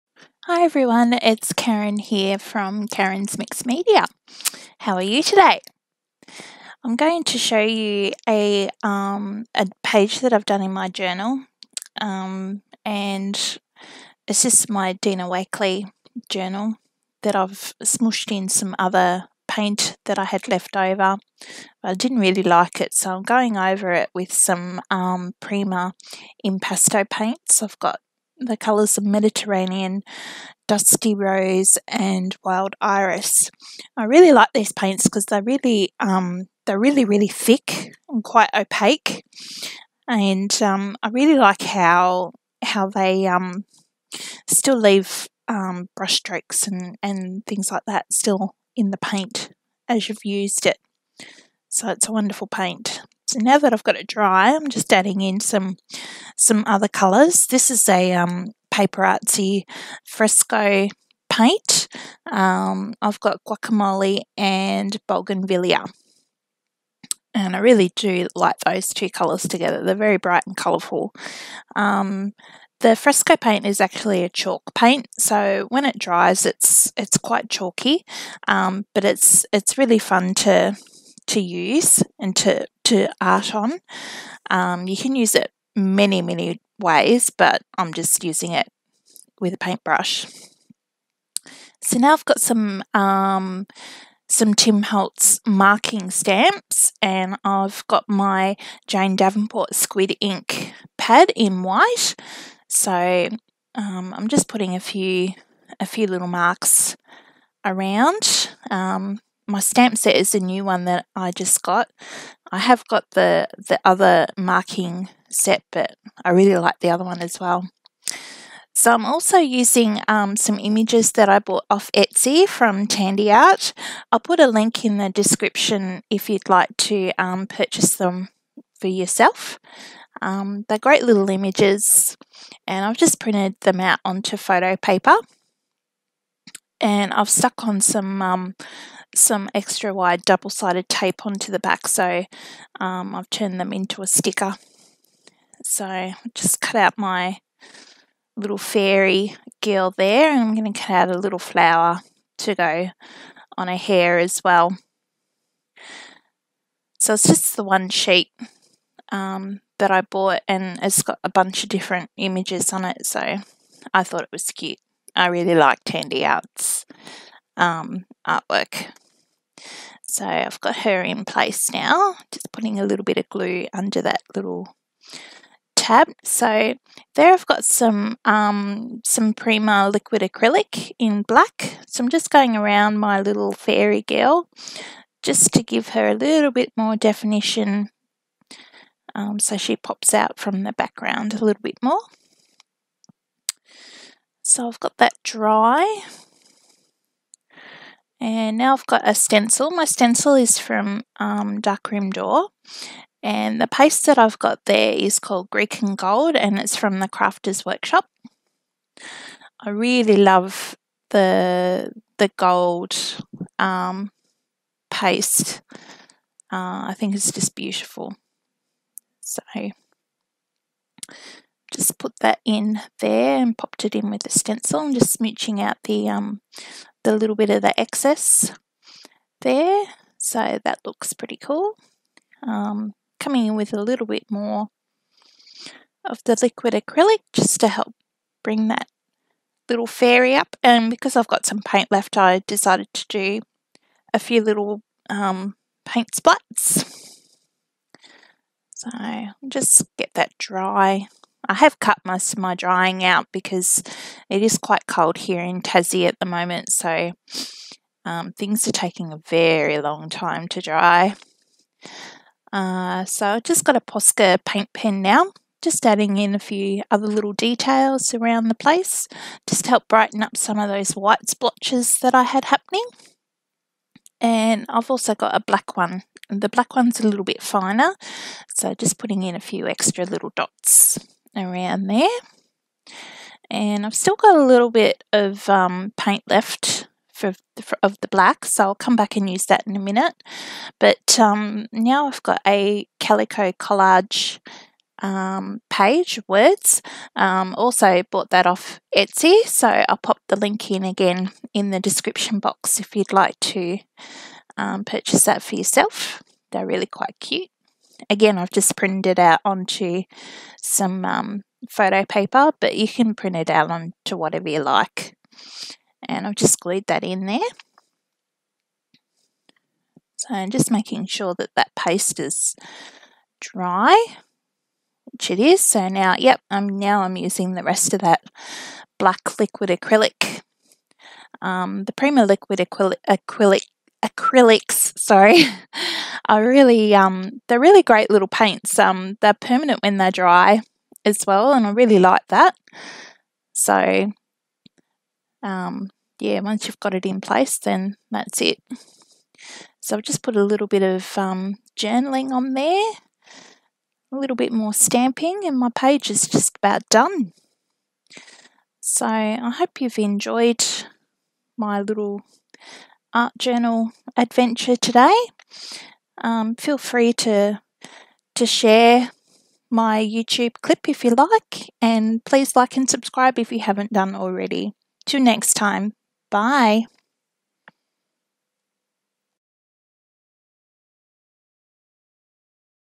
Tandi Art Fairy Page
Art Journal page using acrylic paints, and printouts from Tandi Art and Calio Collage.

Products Used:
Dina Wakley Mini Art Journal
Prima Impasto Acrylic Paint - Mediterranean, Dusty Rose, Wild Iris.
Paper Artsy Fresco Chalk Paint - Bougainvillea and Guacamole
Prima Liquid Acrylic - Ink Black
Jane Davenport Squid Ink - Hybrid ink - Beluga White
Dark Room Door Stencil
TCW Grecian Gold Modeling Paste
Tim Holtz Media marks 2 stamp set

Tandi Art Fairy Story 7: link to Etsy